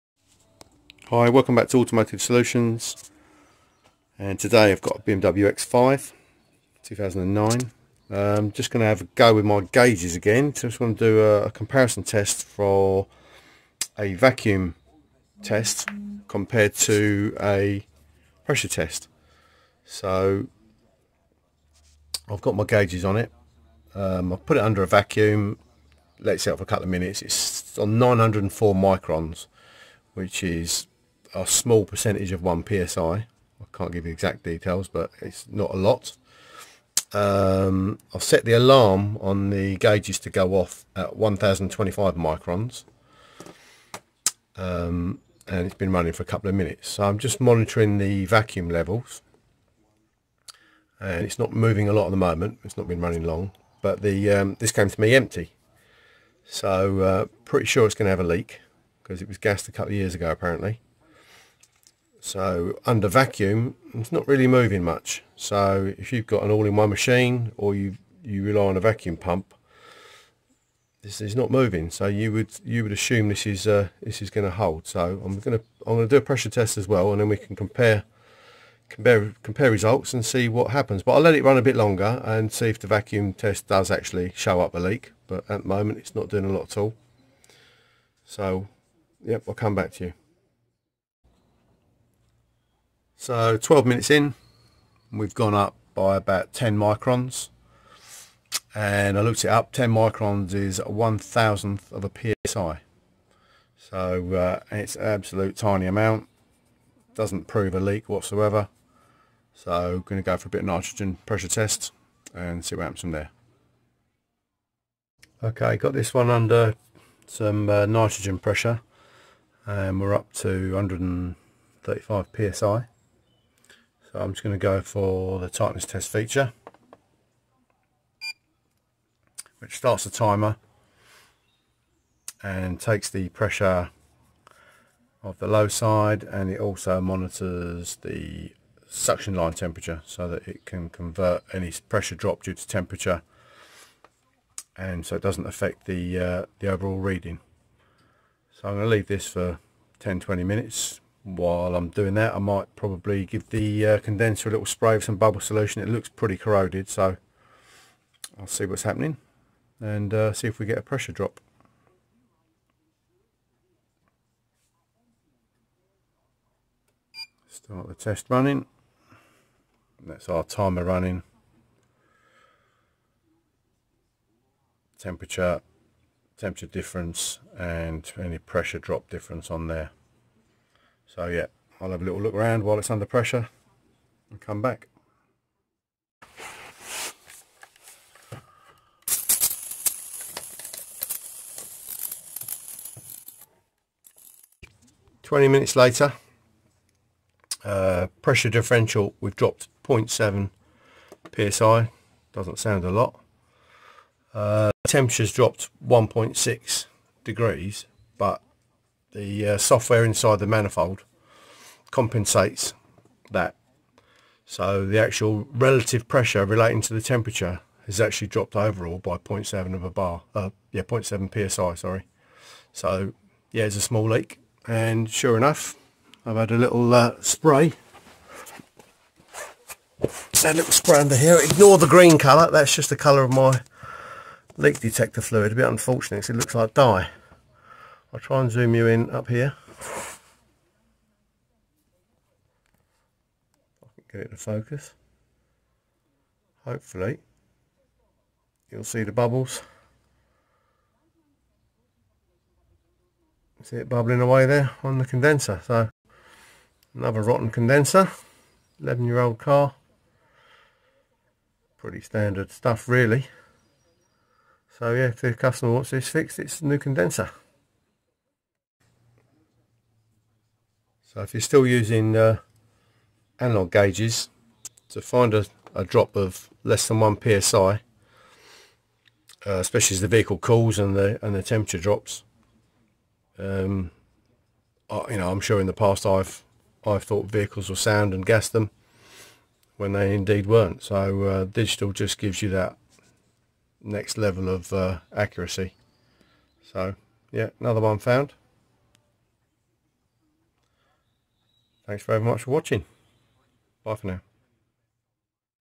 Automotive Air Conditioning - Vacuum and Nitrogen testing.
🙏 if this video helped you then consider sending a bitcoin tip in sats to my Strike (Lightning) wallet
You can use the following address:

There are some things that you need to test Air Conditioning on vehicles properly. One of those is Nitrogen.